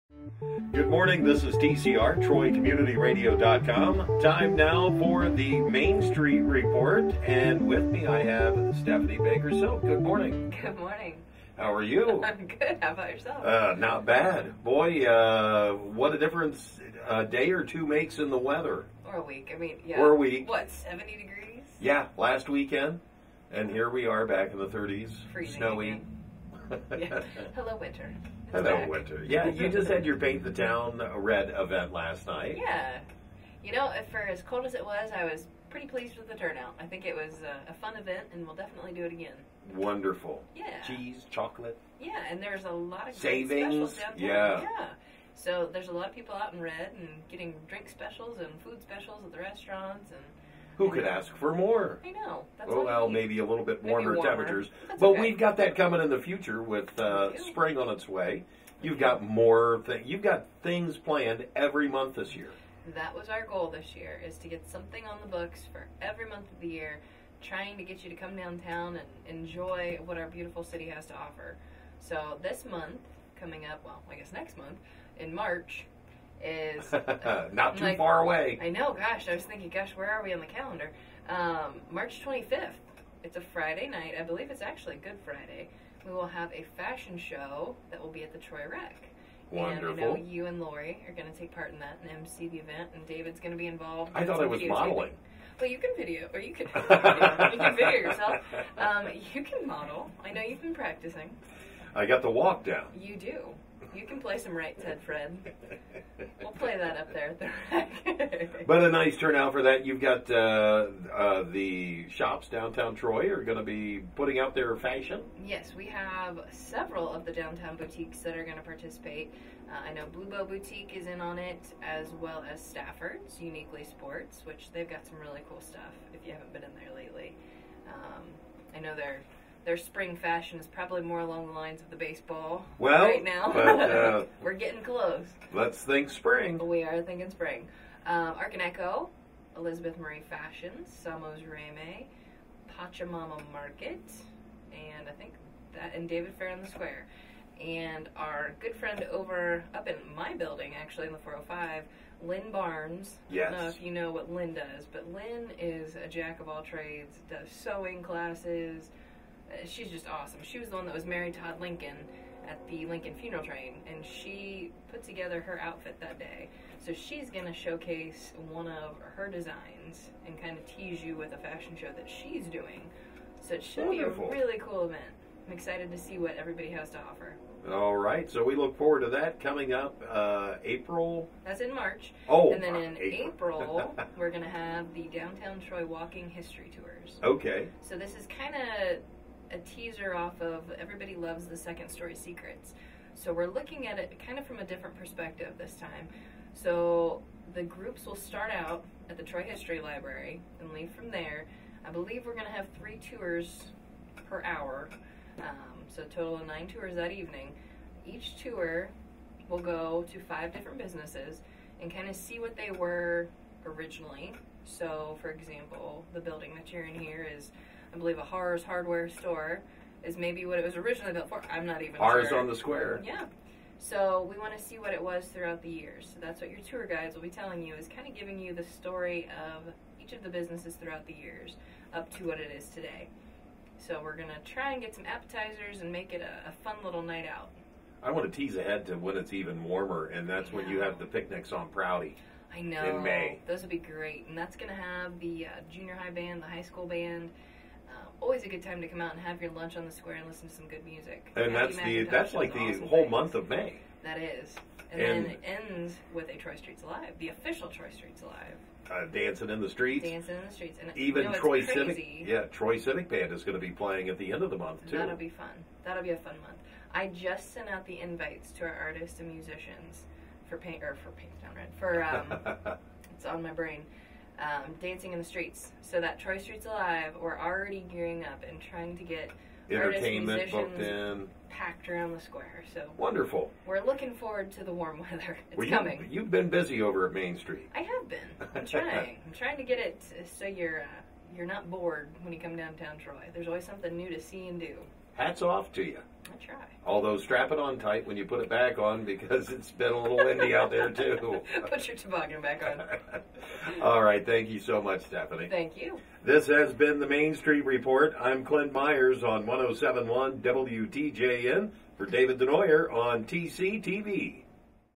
Troy Main St Report (02/27/16)
Paint the Town Red is in the books, so now it's prep time for the Fashion Show coming up in March and Interim Director of Troy Main St, Stephanie Silk has all the details about it in this week's new Troy Main St Report.